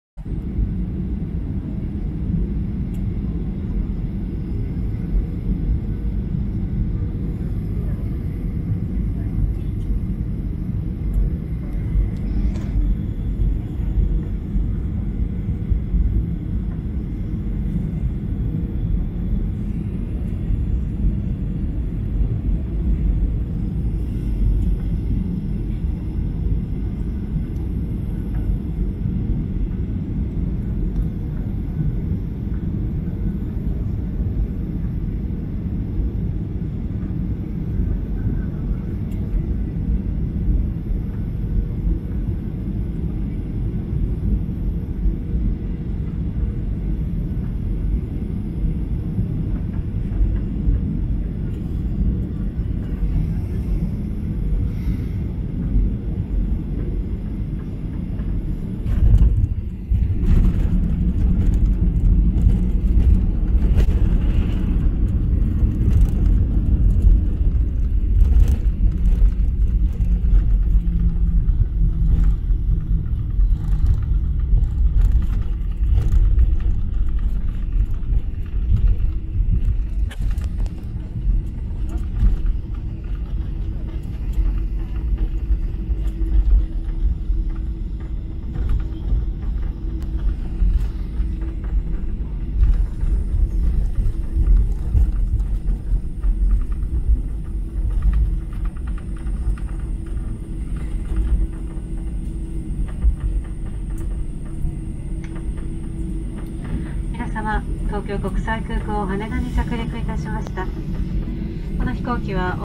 羽田空港同時着陸 なかなかレアかも、2020年初めに撮影。
東京へ旅行に行った時に撮影しました、飛行機に詳しいわけではありませんが、レアならうれしい。フラップが動くところは男のロマン、見入ってしまう。